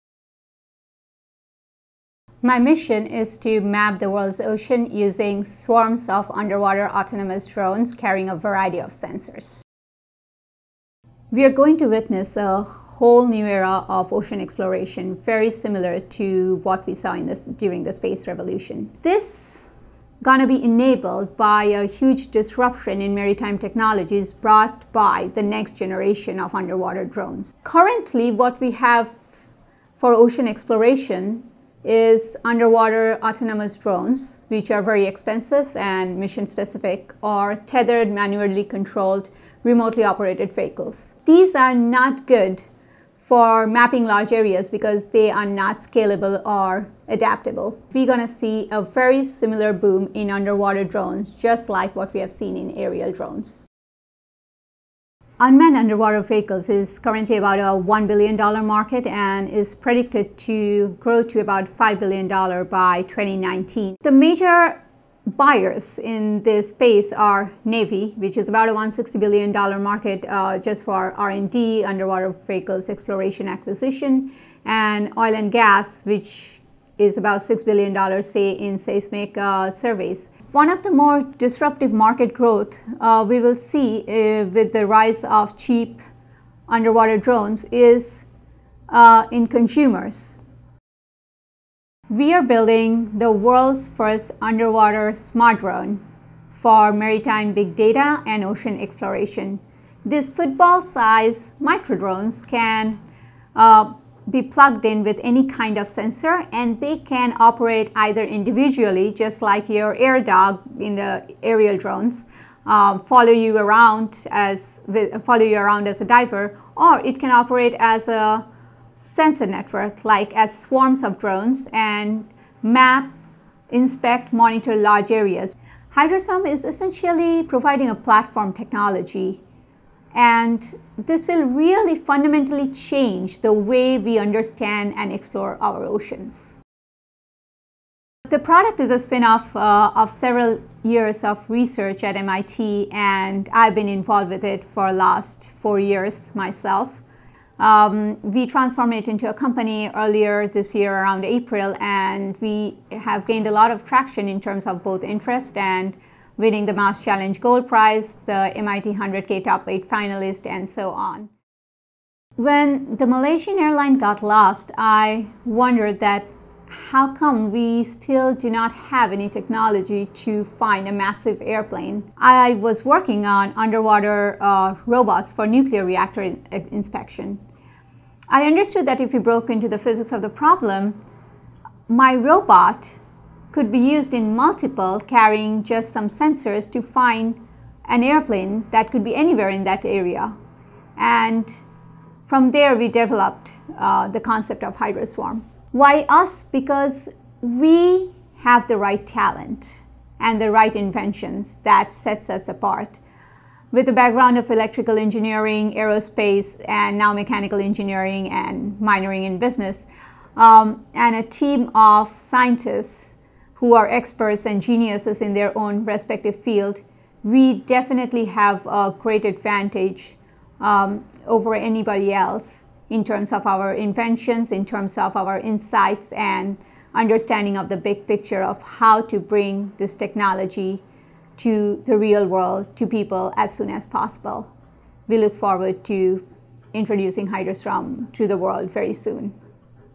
Hydroswarm: Next Generation Drones That Are Exploring The World’s Oceans
Watch Sampriti, founder of the drone startup Hydroswarm, explain how her company's autonomous robots will search and analyze the world's oceans.